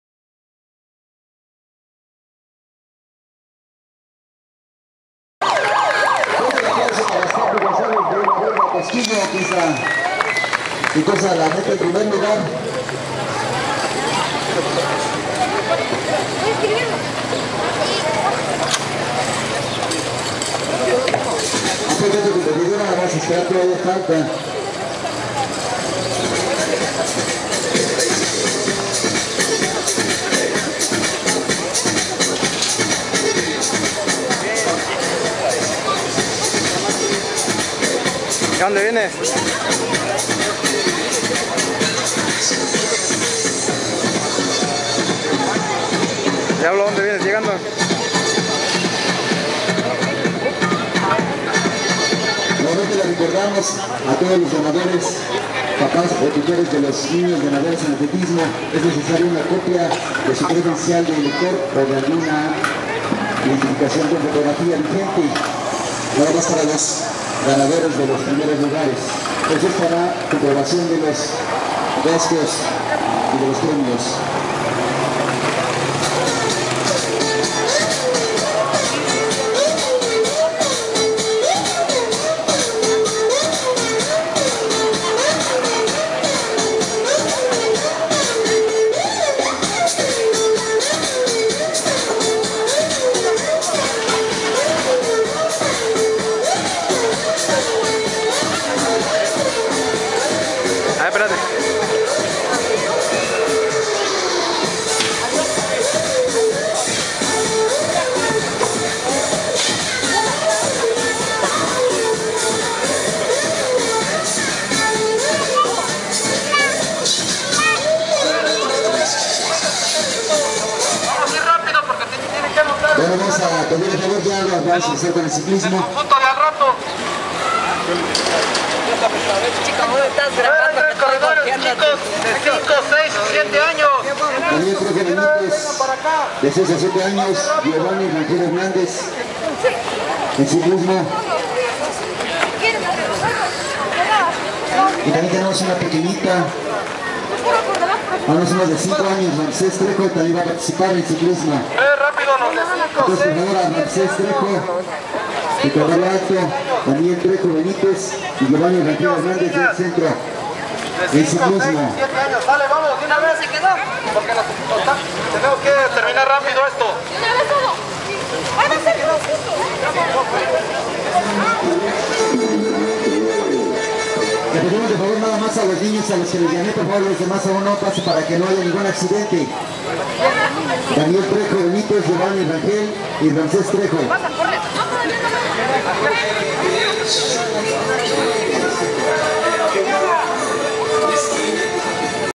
ATLETISMO EN TASQUILLO.HGO EL 15 DE SEPTIEMBRE 2012 CARRERA #3
TASQUILLO HGO 2012
LAS CATEGORIAS FUERON PREESCOLAR,PRIMARIA,SECUNDARIA Y BACHILLER
LAS RAMAS VARONIL Y FEMENIL
ESTE ES EL LINK DONDE ESTAN LAS FOTOS